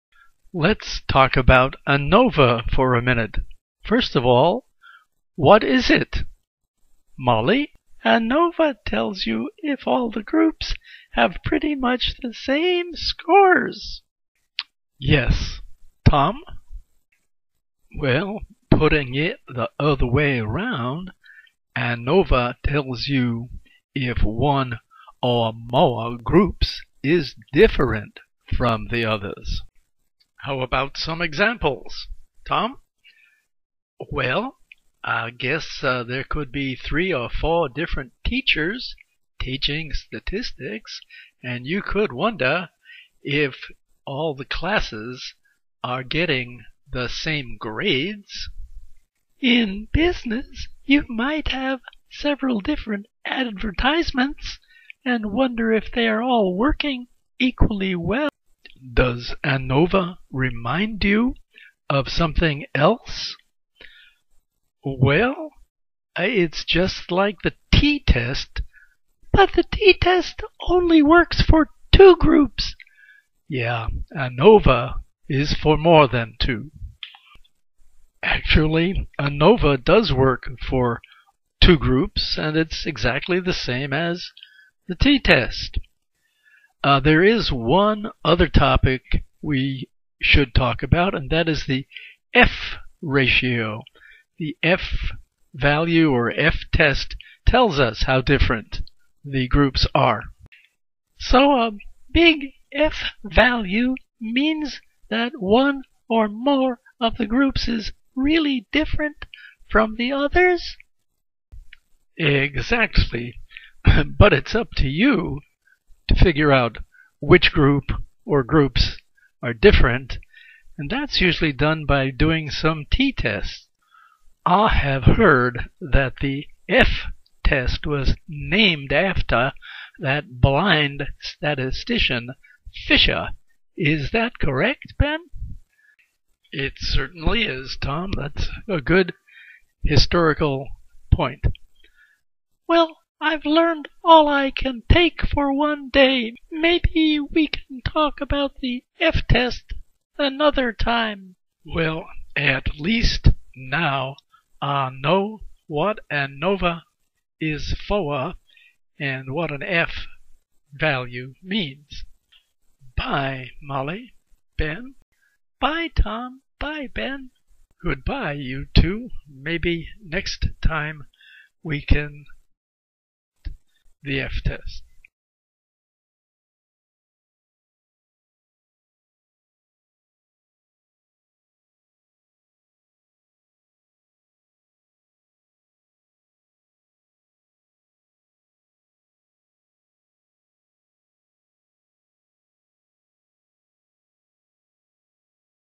Bill Benton Statistics Tutor ANOVA
Explains the basic function of ANOVA statistics.
It also discusses the F-statistic (F-value, F-ratio, F_test). Bill Benton Statistics Tutor ANOVA 7/2012